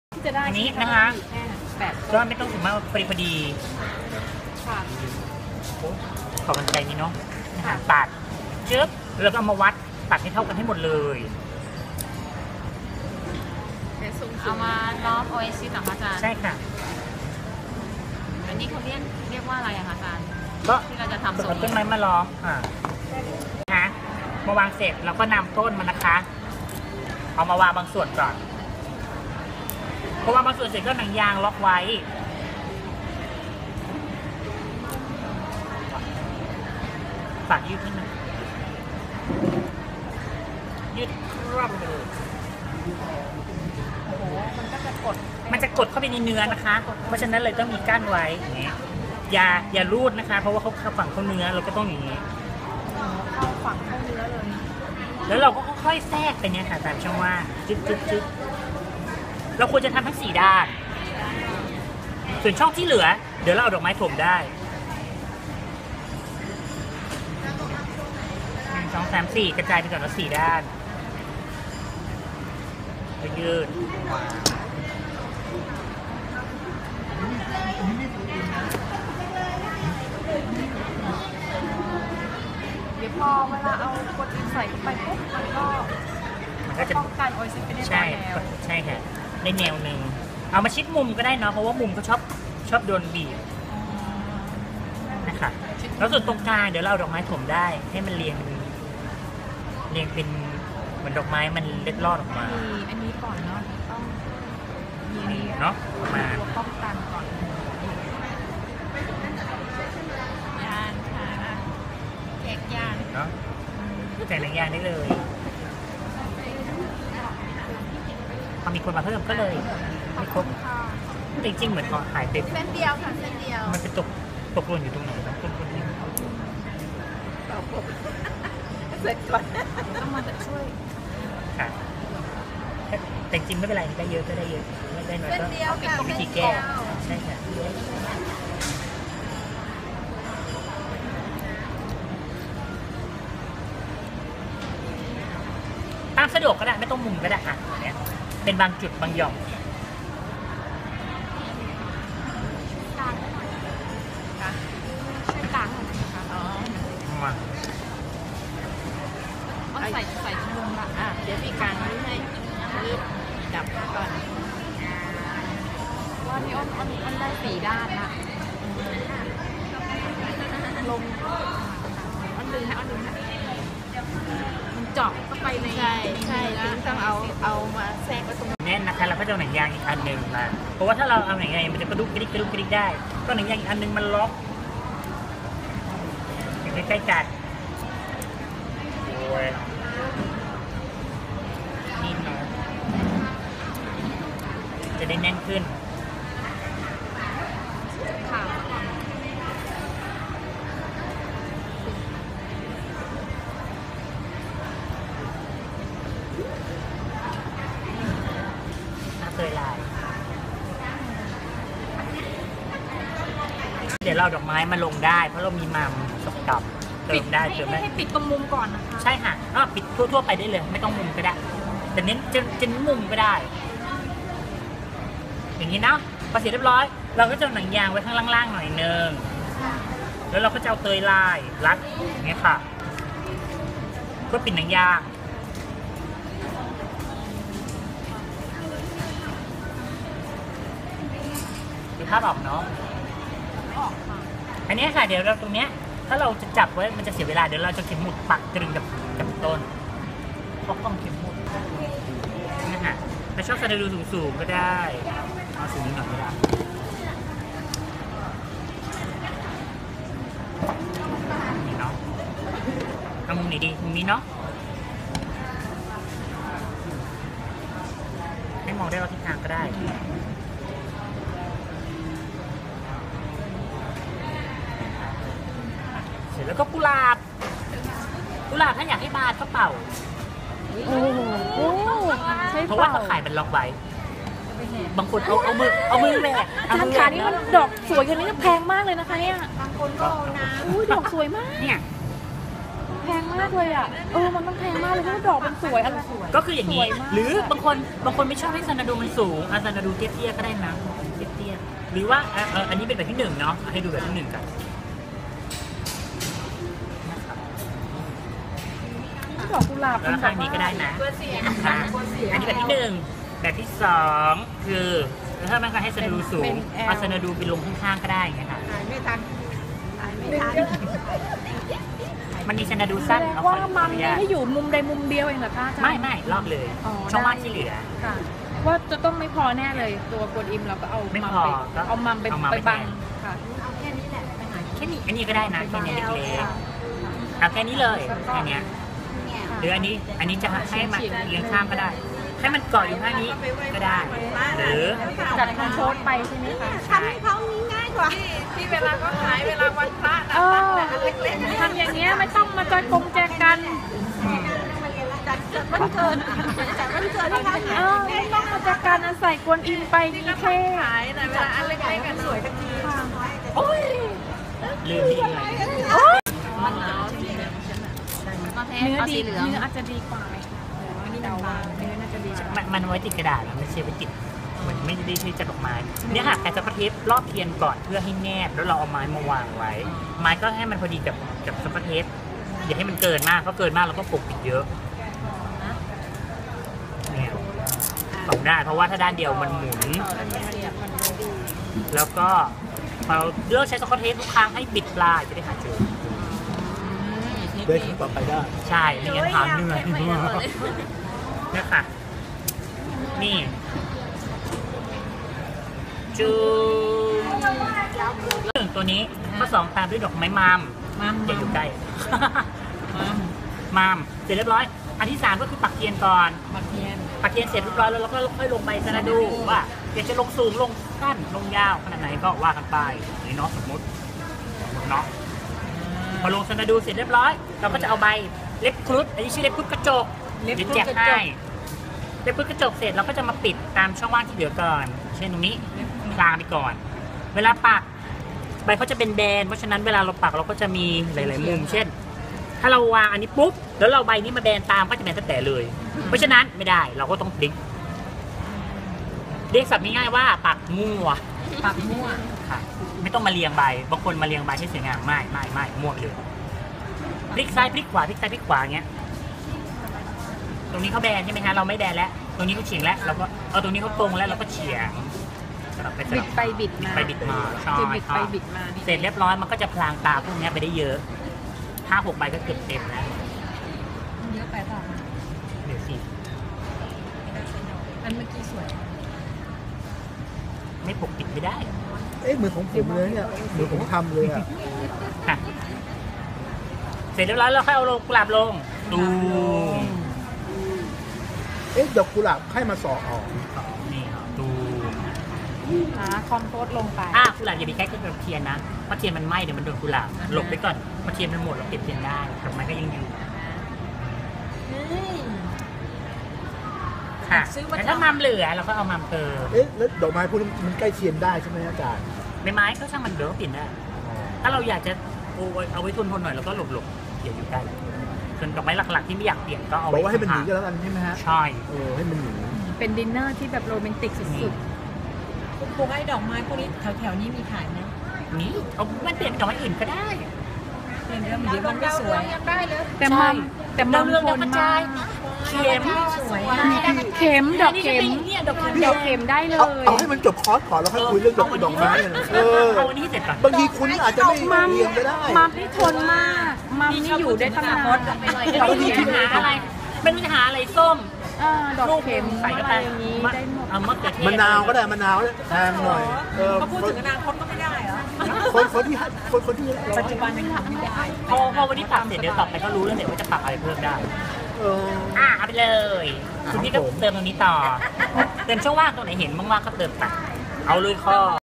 การจัดดอกไม้สดประดับโต๊ะอาหาร แบบที่ 1 โดย อ.ป๊อกกี้ Flowers on the table EP. 6
การจัดดอกไม้สดประดับโต๊ะอาหาร แบบที่ 1 ใช้ใบไม้ร่วมกันหลายชนิดมาประกอบตกแต่ง
สอนโดย อ.กฤตผล  วานิชย์สราพร จากศูนย์ฝึกอาชีพเขตจตุจักร กทม. ณ ห้างยูเนี่ยนมอลล์
อุปกรณ์การทำ ได้แก่ ดอกกุหลาบ, ดอกมัม, ไผ่กวนอิม, ใบซานาดู, ใบเตยลาย, ใบเล็กครูฑกระจก,
โอเอซิส, น้ำ, หนังยาง, ตะปูเข็ม, เทียน, ไม้, มีดคัตเตอร์, กรรไกร